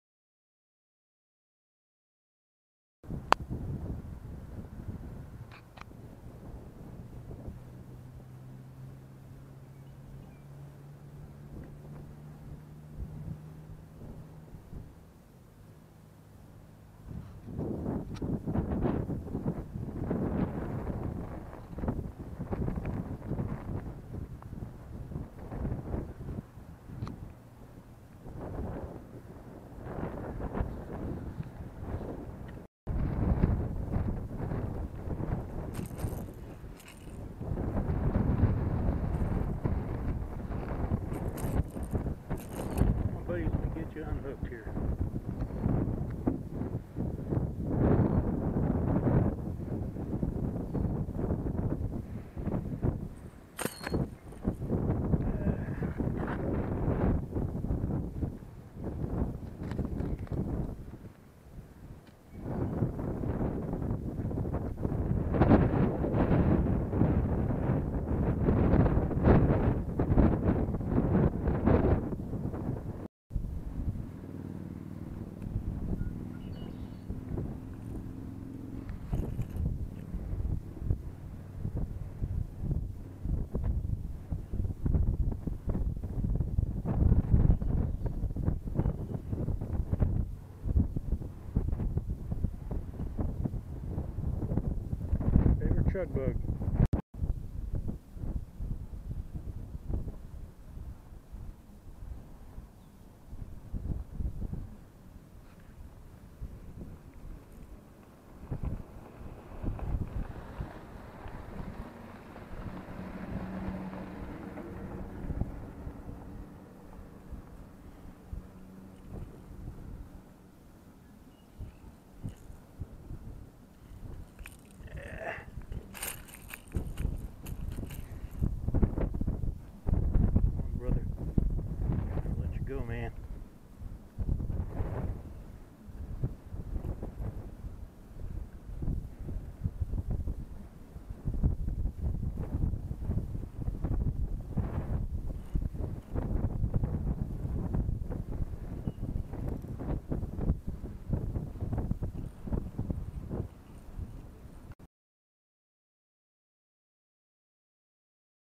Fishing the Ditches East Gator Creek
Fishing the Ditches in East Gator Creek is always productive when the culverts are open